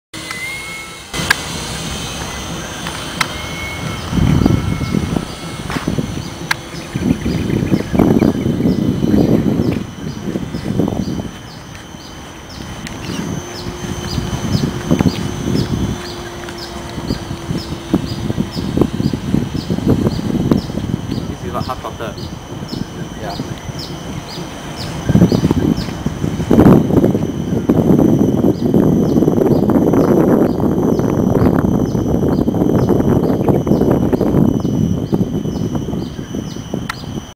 Ian E-hawk2000
Lan Flying e-hawk 2000 at bedok Reservoir